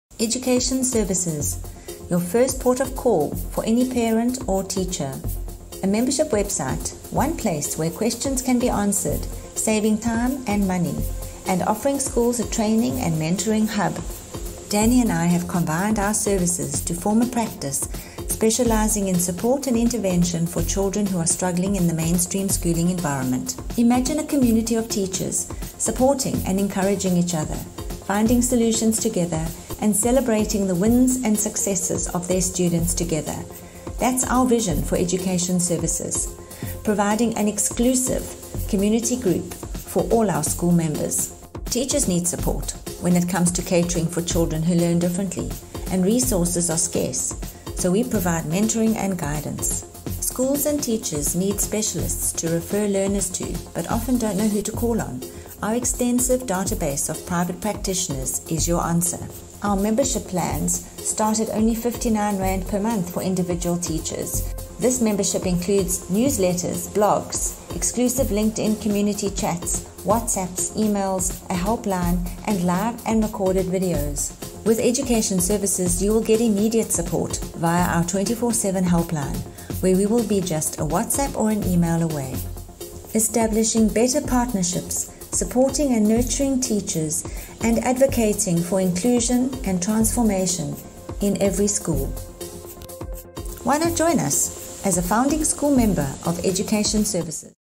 Education Services Introduction Video
This 1.37s video introduces Education Services to teachers and schools.